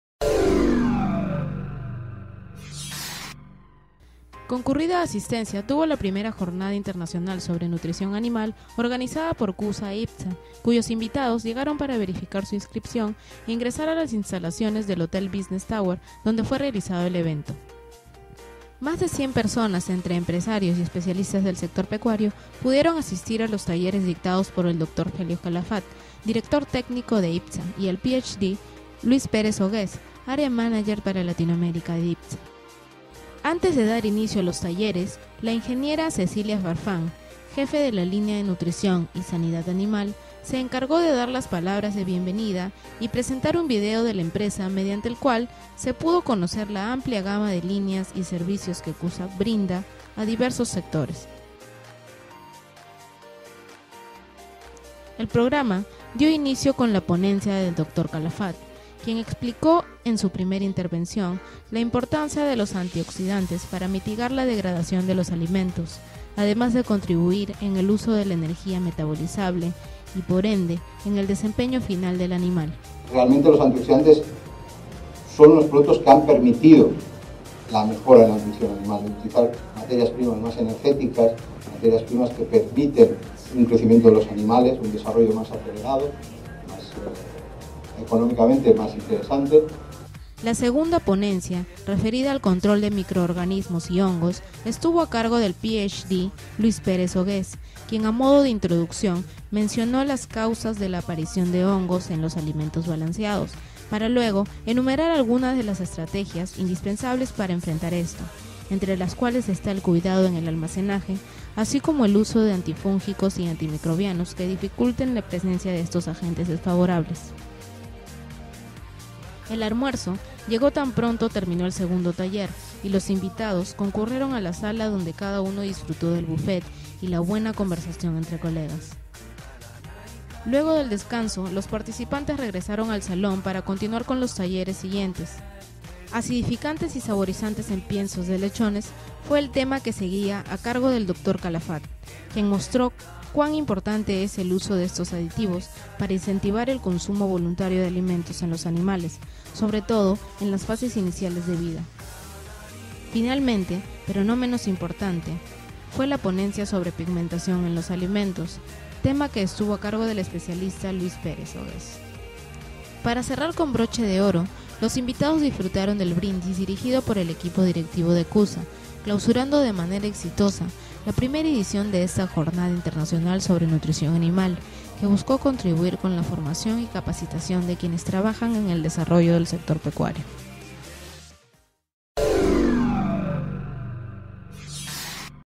CUSA e ITPSA realizaron Jornada Técnica Internacional sobre Nutrición Animal
La Primera Jornada Técnica Integral sobre nutrición animal se llevó a cabo exitosamente, contando con la presencia de más de 100 personas entre empresarios y especialistas relacionados al sector avipecuario, así como con la dirección y organización de los aliados CUSA e ITPSA.

El Dr. Felio Calafat, Director Técnico de ITPSA y el Phd. Luis Pérez Oguez, del Área Manager para Latinoamérica de ITPSA expusieron temas relacionados al control y aditivos para mejorar la alimentación de los animales y así obtener una mejora notable en la producción.

Tópicos como la importancia de los antioxidantes para prevenir el deterioro de los alimentos y el uso de acidificantes y saborizantes para inducir la alimentación en los animales fueron abordados por el Dr. Calafat en una ponencia más que instructiva.

Mientras que el Ph.D. Luis Pérez, expuso sobre al control de hongos, virus y micotoxinas que dañan el almacenaje y la calidad de los comestibles balanceados. Asimismo, su ponencia final estuvo referida con el uso de pigmentantes para mejorar las características de los productos que se ofrecen.

Para finalizar los asistentes pasaron al respectivo salón donde se realizó el brindis tradicional con la presencia de los principales integrantes del Directorio de CUSA e ITPSA.